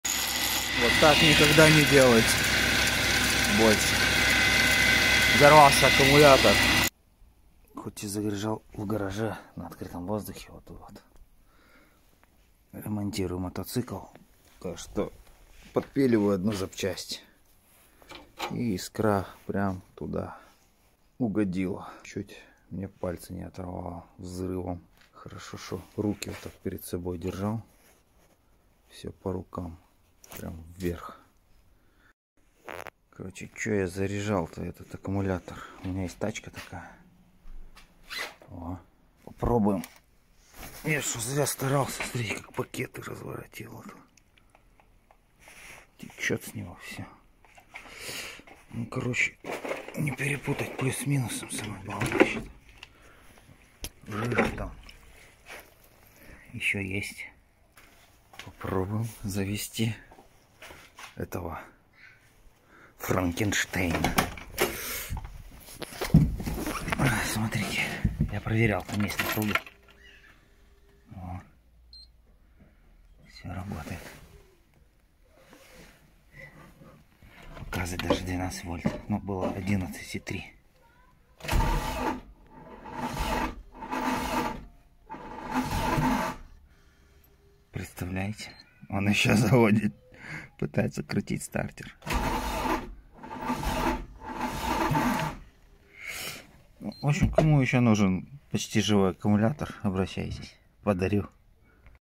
Взорвался аккумулятор
От искры взорвался аккумулятор на зарядке. Он еще работает после взрыва.
Взрыв был мощным как здоровая питарда. Повредил пальцы.